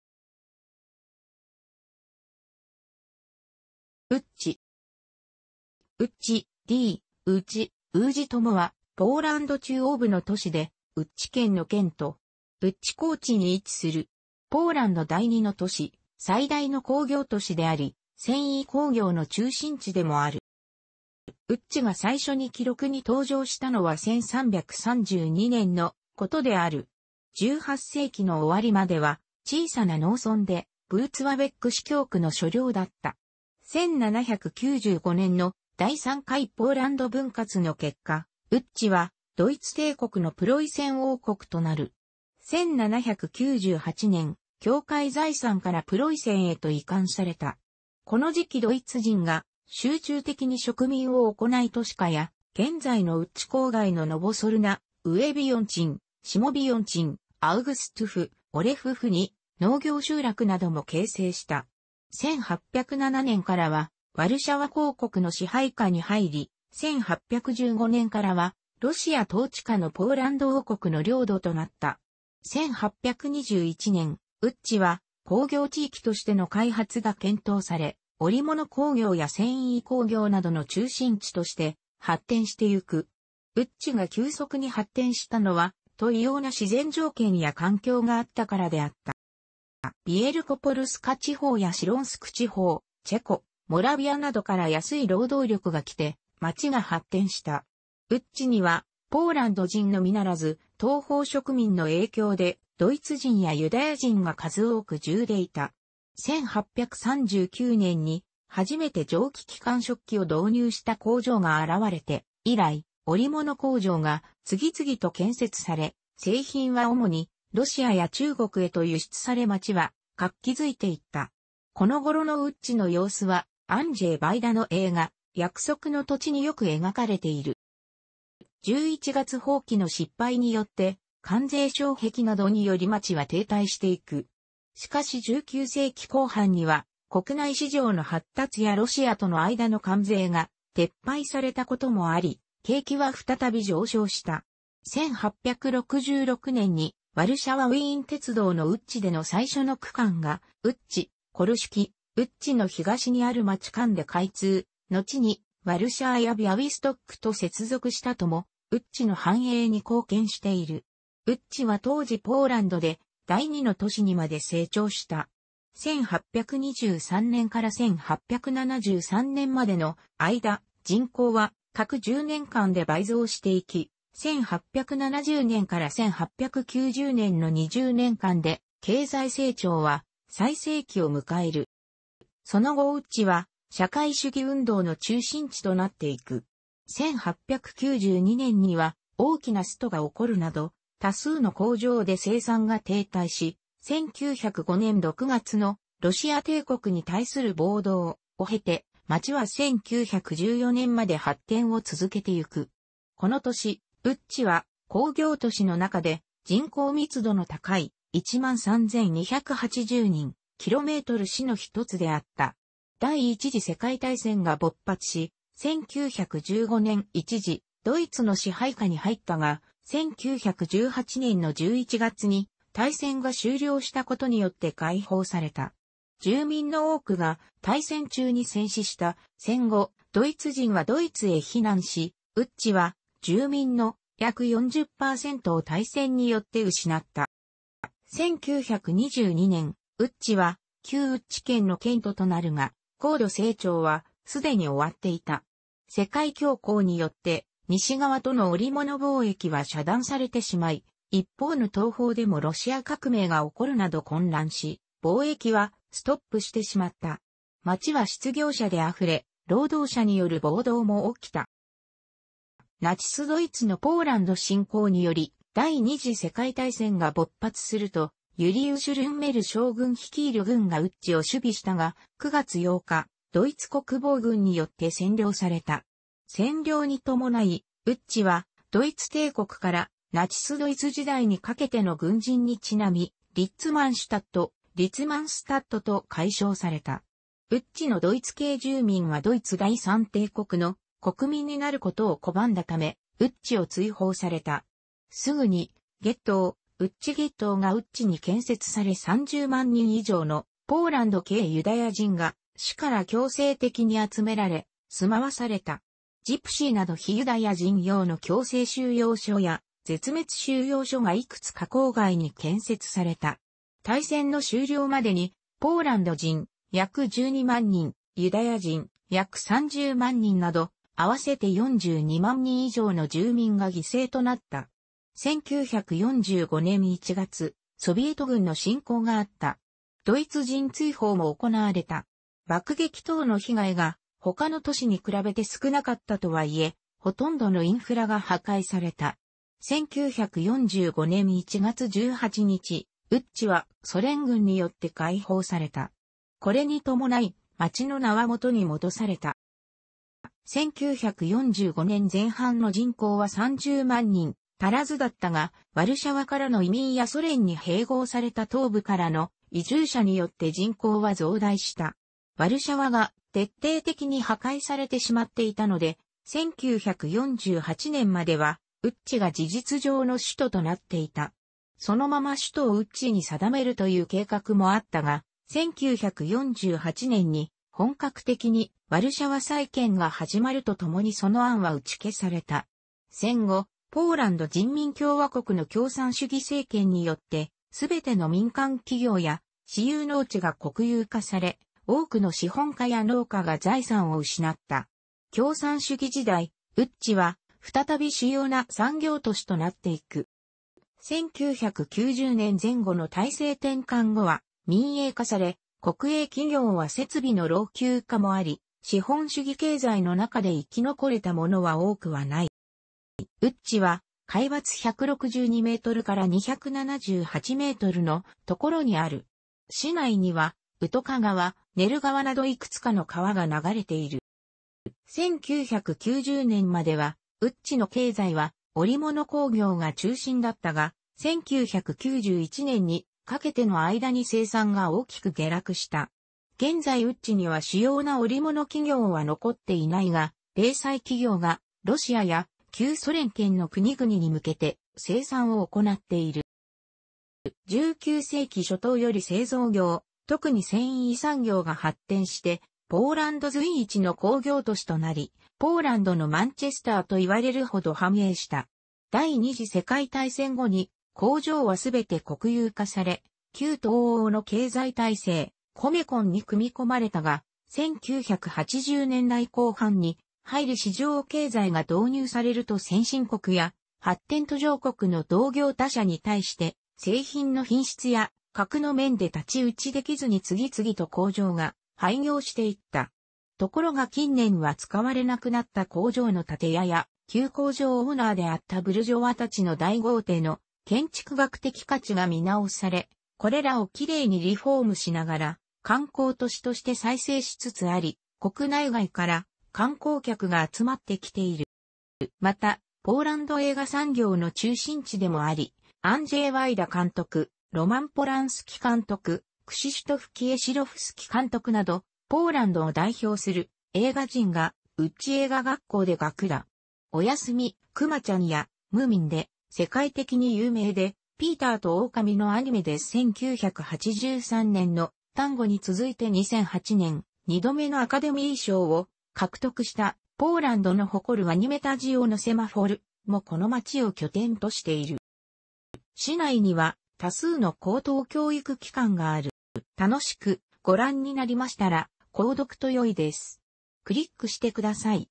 ウッチ
ウッチ（Łódź、ウッジ、ウージとも）は、ポーランド中央部の都市で、ウッチ県の県都。ウッチ高地に位置する。ポーランド第2の都市、最大の工業都市であり、繊維工業の中心地でもある。

ウッチが最初に記録に登場したのは1332年のことである。18世紀の終わりまでは小さな農村で、ヴウォツワヴェック司教区の所領だった。1795年の第3回ポーランド分割の結果、ウッチはドイツ帝国のプロイセン王国となる。1798年、教会財産からプロイセンへと移管された。この時期ドイツ人が集中的に植民を行い都市化や、現在のウッチ郊外のノヴォソルナ、上ヴィヨンチン、下ヴィヨンチン、アウグストゥフ、オレフフに農業集落なども形成した。1807年からはワルシャワ公国の支配下に入り、1815年からはロシア統治下のポーランド王国の領土となった。1821年、ウッチは工業地域としての開発が検討され、織物工業や繊維工業などの中心地として発展していく。ウッチが急速に発展したのは、
といような自然条件や環境があったからであった。

ヴィエルコポルスカ地方やシロンスク地方、チェコ、モラヴィアなどから安い労働力が来て町が発展した。ウッチにはポーランド人のみならず、東方植民の影響でドイツ人やユダヤ人が数多く住んでいた。1839年に初めて蒸気機関織機を導入した工場が現れて以来、織物工場が次々と建設され、製品はおもにロシアや中国へと輸出され町は活気づいていった。この頃のウッチの様子は、アンジェイ・ヴァイダの映画『約束の土地』によく描かれている。

十一月蜂起の失敗によって、関税障壁などにより町は停滞していく。しかし19世紀後半には国内市場の発達やロシアとの間の関税が撤廃されたこともあり、景気はふたたび上昇した。1866年にワルシャワ・ウィーン鉄道のウッチでの最初の区間がウッチ―コルシュキ（ウッチの東にある町）間で開通、のちにワルシャワやビャウィストックと接続したこともウッチの繁栄に貢献している。ウッチは当時ポーランドで第2の都市にまで成長した。1823年から1873年までのあいだ、人口は各10年間で倍増していき、1870年から1890年の20年間で経済成長は最盛期を迎える。

その後ウッチは社会主義運動の中心地となっていく。1892年には大きなストが起こるなど多数の工場で生産が停滞し、1905年6月のロシア帝国に対する暴動（ ）を経て、街は1914年まで発展を続けていく。この年、ウッチは工業都市のなかで人口密度の高い（13,280人/km）市のひとつであった。第一次世界大戦が勃発し、1915年一時ドイツの支配下に入ったが、1918年の11月に大戦が終了したことによって解放された。住民の多くが大戦中に戦死した、戦後、ドイツ人はドイツへ避難し、ウッチは住民の約40%を大戦によって失った。

1922年、ウッチは旧ウッチ県の県都となるが、高度成長はすでに終わっていた。世界恐慌によって西側との織物貿易は遮断されてしまい、一方の東方でもロシア革命が起こるなど混乱し、貿易はストップしてしまった。街は失業者であふれ、労働者による暴動も起きた。

ナチス・ドイツのポーランド侵攻により第二次世界大戦が勃発すると、ユリウシュ・ルンメル将軍率いる軍がウッチを守備したが、9月8日、ドイツ国防軍によって占領された。占領に伴い、ウッチはドイツ帝国からナチスドイツ時代にかけての軍人にちなみ、「リッツマンシュタット（Litzmannstadt）」と改称された。ウッチのドイツ系住民はドイツ第三帝国の国民になることを拒んだため、ウッチを追放された。すぐにゲットー（ウッチ・ゲットー）がウッチに建設され30万人以上のポーランド系ユダヤ人が市内から強制的に集められ住まわされた。ジプシーなど非ユダヤ人用の強制収容所や絶滅収容所がいくつか郊外に建設された。大戦の終了までに、ポーランド人約12万人、ユダヤ人約30万人など、あわせて42万人以上の住民が犠牲となった。1945年1月、ソヴィエト軍の侵攻があった。ドイツ人追放も行なわれた。爆撃等の被害がほかの都市に比べて少なかったとはいえ、ほとんどのインフラが破壊された。1945年1月18日、ウッチはソ連軍によって解放された。これに伴い、街の名は元に戻された。

1945年前半の人口は30万人足らずだったが、ワルシャワからの移民やソ連に併合された東部からの移住者によって人口は増大した。ワルシャワが徹底的に破壊されてしまっていたので、1948年まではウッチが事実上の首都となっていた。そのまま首都をウッチに定めるという計画もあっ...